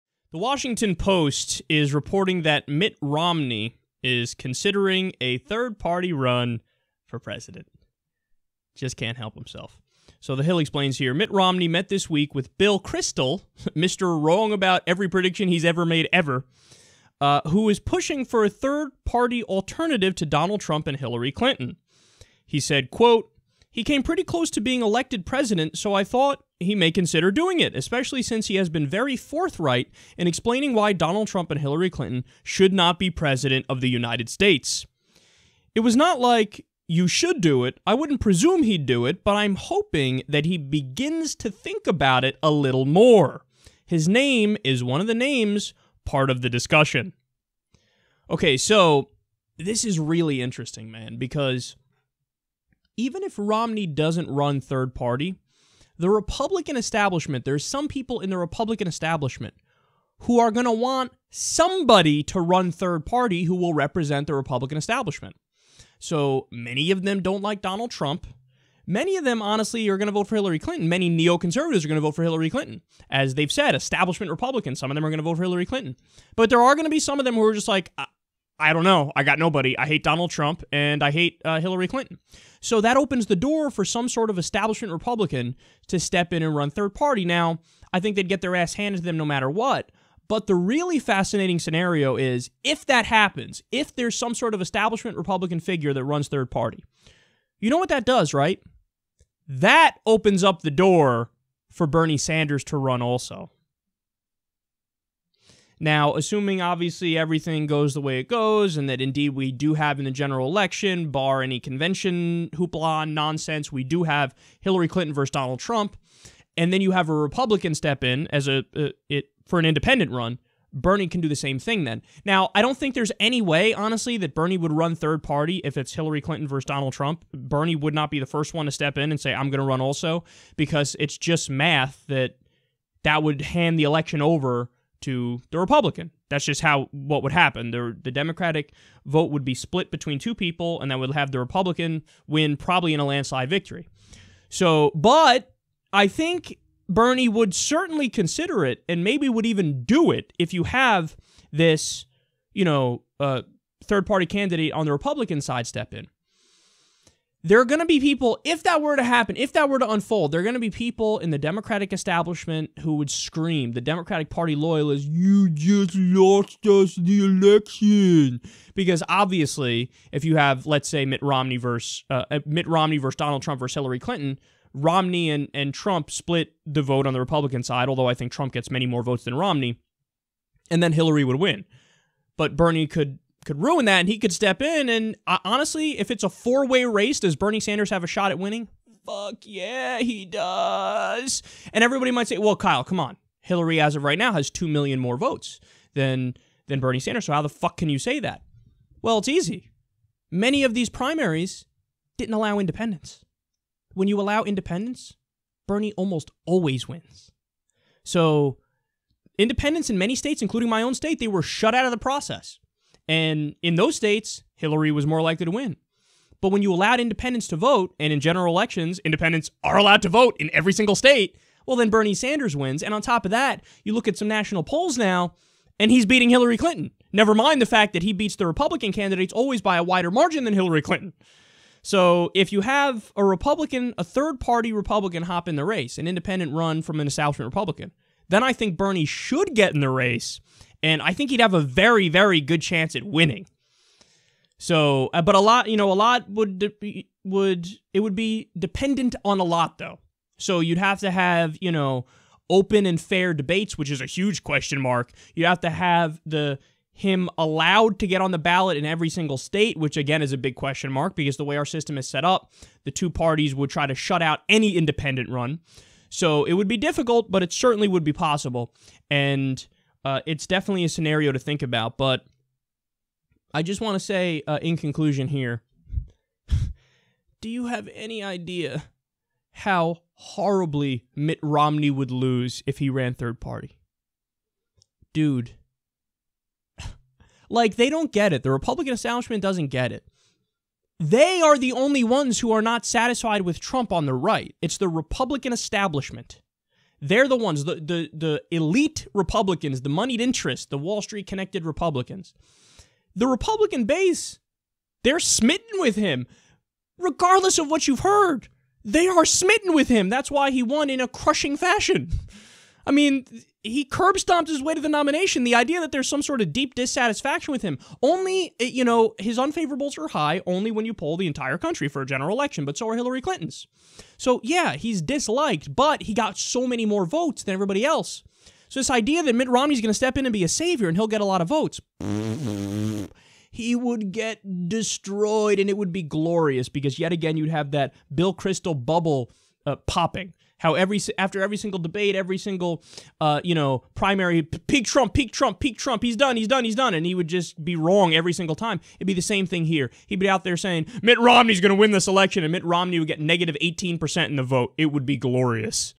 Mitt Romney Considering Third Party Run
Mitt Romney met this week with William Kristol, who is pushing for a third-party alternative to Donald Trump and Hillary Clinton.

“He came pretty close to being elected president so I thought he may consider doing it, especially since he has been very forthright in explaining why Donald Trump and Hillary Clinton should not be president of the United States,” Kristol told the Washington Post Friday...